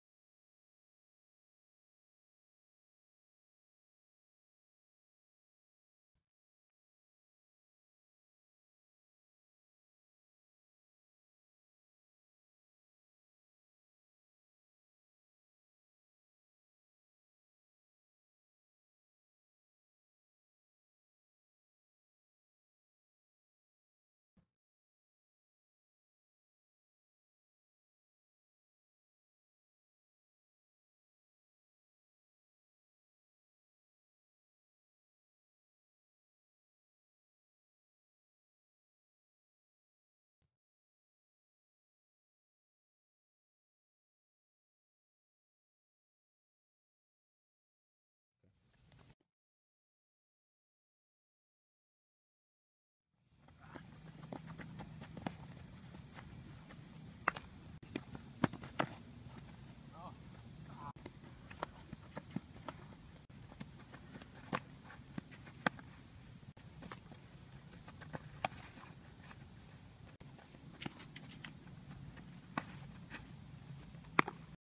Tanner Anderson
Class of 2017. SS/OF. Switch Hitter. 6.9 in the 60. ACT: 28 Lots of athleticism. Great instincts. Great attitude. Reliable, Respectful, Trustworthy.